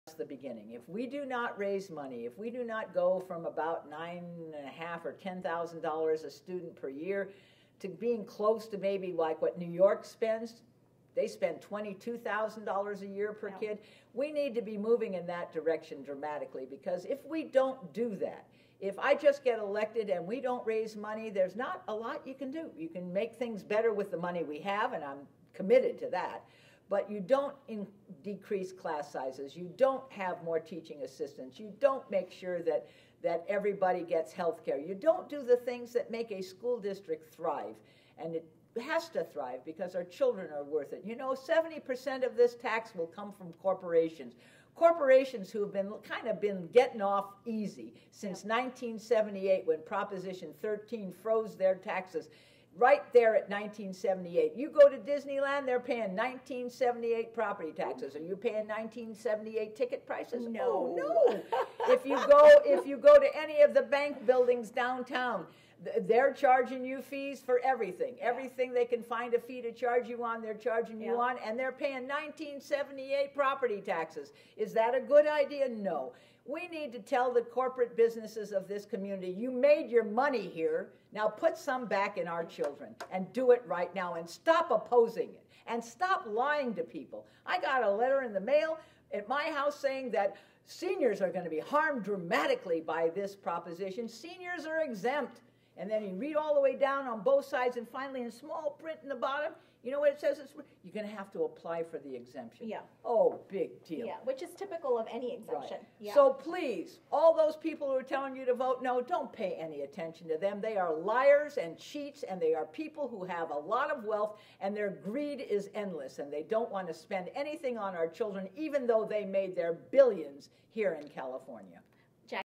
LA School Voice — School funding measure — Jackie Goldberg
Jackie Goldberg has spent her life fighting for students. Now she's ready to go to war to save public education.

Vote Yes on Measure EE.
It's for our students and our city.